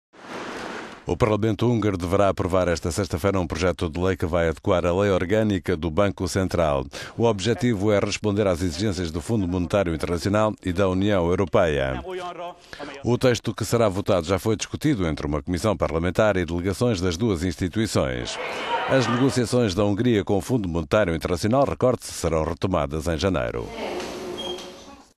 Hungria abre portas ao FMI
O Parlamento húngaro deverá aprovar esta sexta-feira, um projecto de lei que vai alterar a lei orgânica do Banco Central.

O objetivo é responder às exigências do Fundo Monetário Internacional e da União Europeias.

O texto que será votado já foi discutido entre uma comissão parlamentar e delegações das duas instituições.

As negociações da Húngria com o Fundo Monetário Internacional - recorde-se - serão retomadas em Janeiro